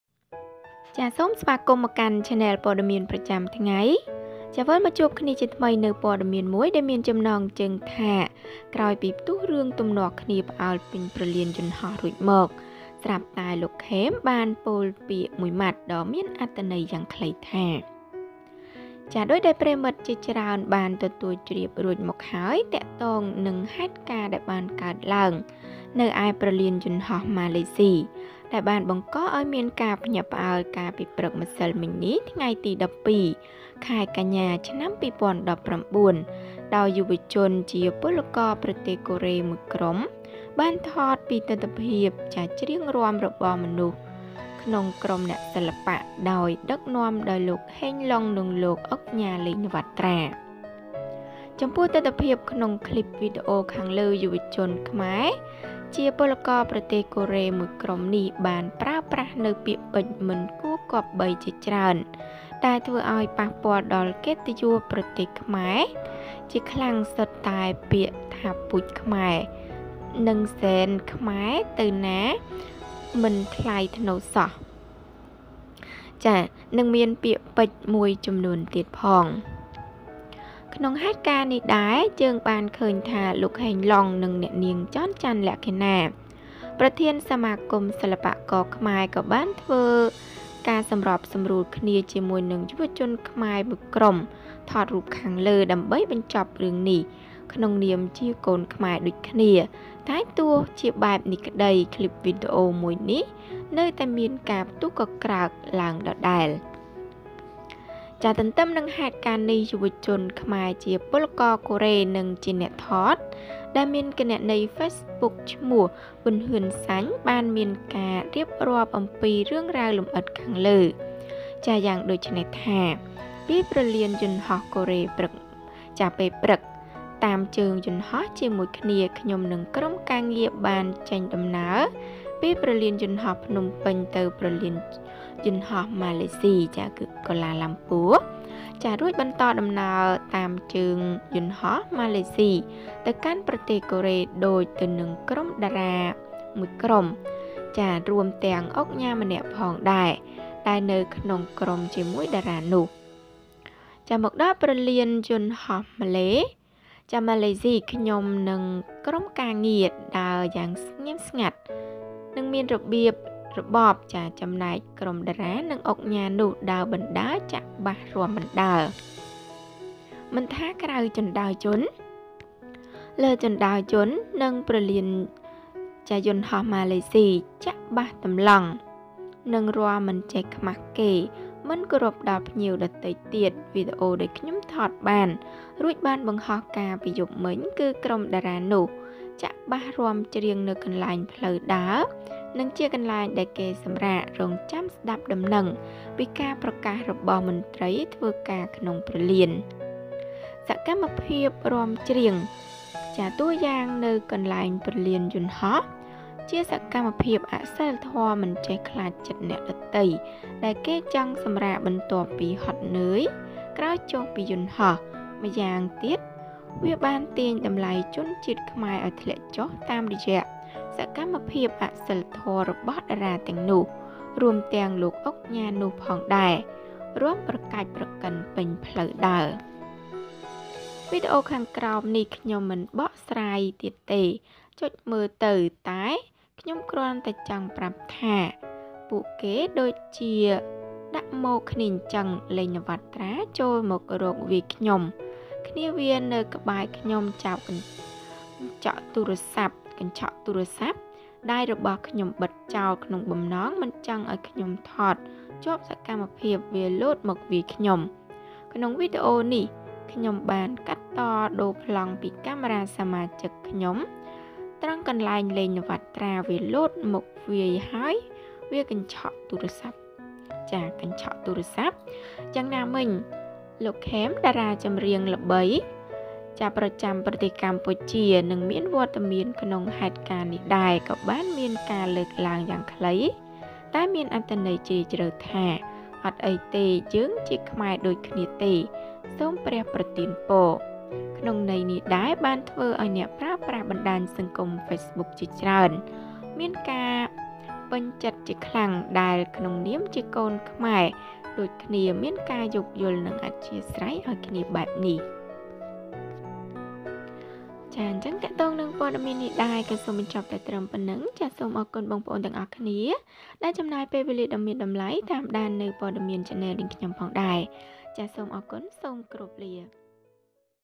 ក្រោយពីផ្ទុះរឿងទំនាស់គ្នាផ្អើលពេញព្រលានយន្តហោះរួចមក ស្រាប់តែលោក ខេម បានពោលពាក្យមួយម៉ាត់ដ៍មានអត្ថន័យ
After a series of explosions over the airport, Cam said one word: "

Thank You So much !